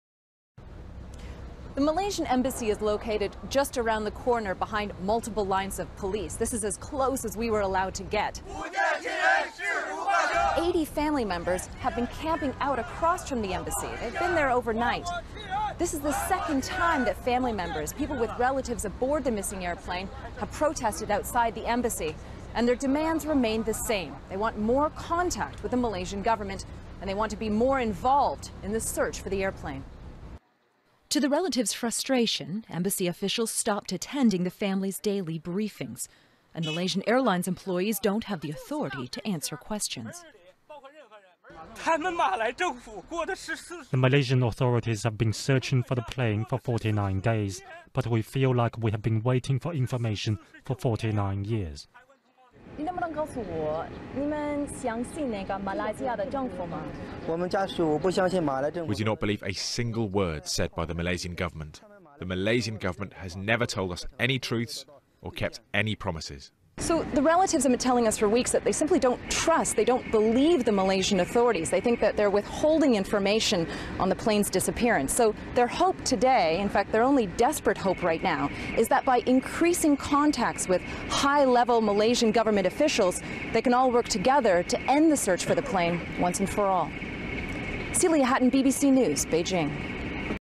Missing Malaysian plane: Relatives in China vent anger
Almost seven weeks have passed since flight MH370 disappeared en route to Beijing.

The relatives of many of the missing Chinese passengers have spent that time at a Beijing hotel, anxiously waiting for information.

But last night, they left the hotel en masse and marched to the nearby Malaysian Embassy. They are now refusing to leave the area outside the embassy gates.

Our correspondent Celia Hatton explains what sparked their anger.